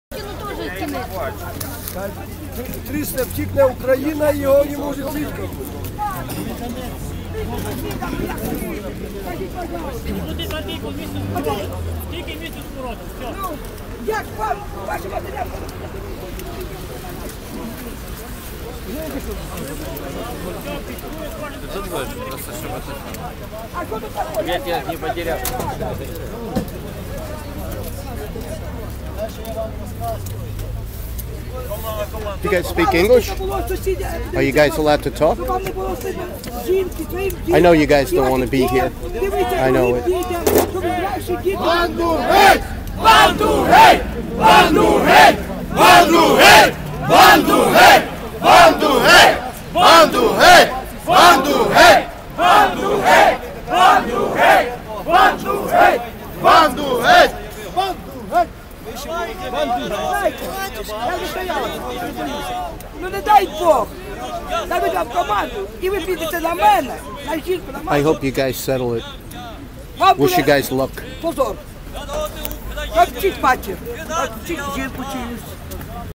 Protests in Kiev Ukraine Monday December 9th 2013 14:03
watch as I go through the ranks of pretend protesters in Kiev Ukraine, I spoke directly with the police , people were posing for photos for the foreign press it was totally disgusting none of the press were video taping , I went through the ranks, and the pretend protesters started chanting "The President must go away " in Ukrainian
I noticed the Police on the front lines were all babyfaced kids ..and they understood me, and did NOT want to be there .one looked like he has tears welling up in his eyes Old women, old men and pretend protesters were abusing these police men

I call them pretend protesters because NONE knew that the European Union is actually a very Soviet like entity , the people they would vote for DO NOT MAKE THE LAWS ...banker and Corporate persons that are exempt from taxation and laws everyone else is subject too. I saw and spoke with many teens and others , they are clueless to the political powderkeg they have made